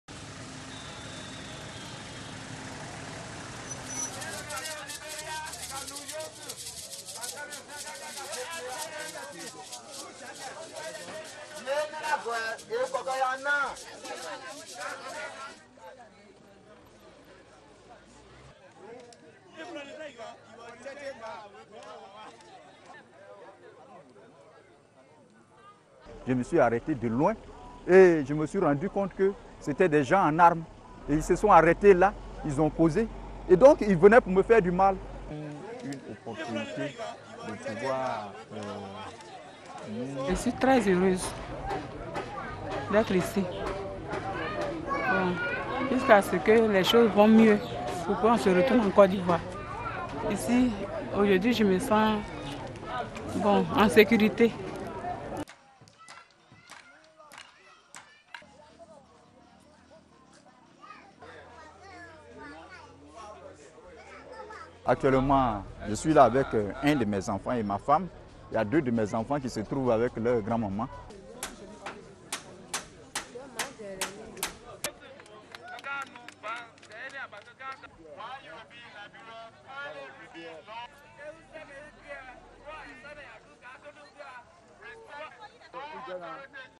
MaximsNewsPEOPLE: LIBERIA -- 15,000 IVORIAN REFUGEES -- UN REFUGEE AGENCY
MaximsNewsPEOPLE:08 March 2011 -- UNHCR: UN Refugee Agency -- Liberia - Three months after these refugees fled their country, Cote d'Ivoire, they arrive to their temporary home, Camp Bahn in Liberia; the journey from the village of Luguanto, where they were living with local families, took over five hours. According to UNHCR staff, refugees entering Liberia said they fled actual fighting between the Forces Nouvelles and the forces loyal to Laurent Gbagbo. The worsening situation in Côte d'Ivoire could point to a return to civil war.

They are first families to arrive in the new camp, all fleeing the post election violence which has gripped their country.

Bahn refugee camp was set up to deal with the influx. Philippe Cheugui, an Ivorian school teacher, was a supporter of Laurent Bagbo, the incumbent who refuses to give up his presidency.

Cheugui came home one night only to be threatened. Elisée, his wife, was a shop owner in Cote d'Ivoire.

Initially villages hosted the refugees, but it was a struggle to feed and accommodate them.

Camp Bahn will alleviate some of that need, but it can take in only 15 thousand. In the last few weeks, the numbers of new arrivals has surged to more than 55 thousand, most from western Côte d'Ivoire.

They too need shelter. Philippe is grateful for the safety he and his wife have found, but he is still worried. The fighting has escalated in Côte d'Ivoire, so Philippe is anxious to be reunited with his daughters.

SOUNDBITE (French) Philippe Cheugui, teacher, Ivorian Refugee:
"I stop from afar and I realized that there were armed people. They were stopped there and they had come to hurt me."
SOUNDBITE (French) Elisée Veh Gueudoua, shop owner, Ivorian Refugee:
"I am really happy to be here, until things get better. Then we can go back to Côte d'Ivoire. I am just happy to feel secure."

"Actually, I came with one of my children and my wife. I have two children who are there with their grandmother."